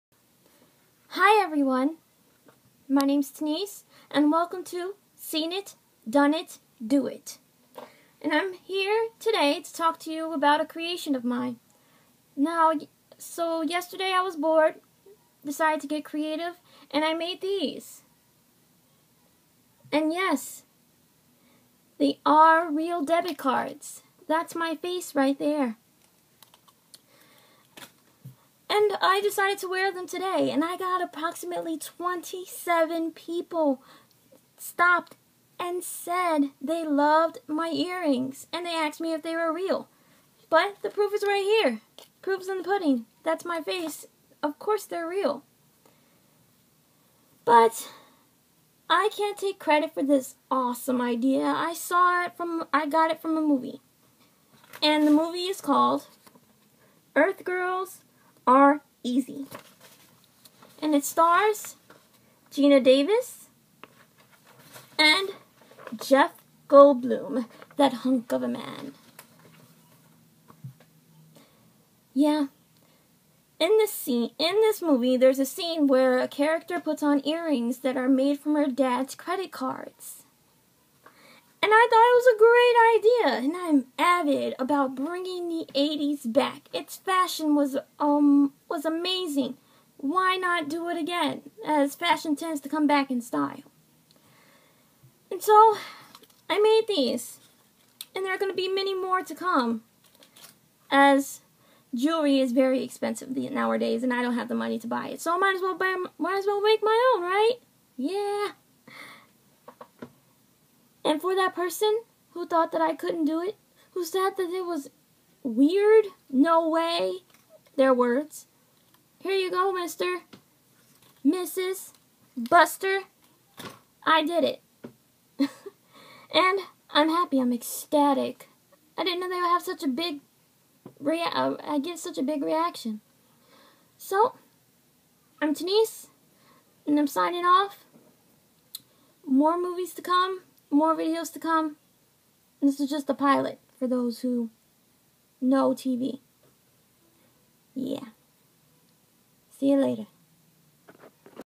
Seen It, Done it, Do It.
Created unique jewelry inspired by movie.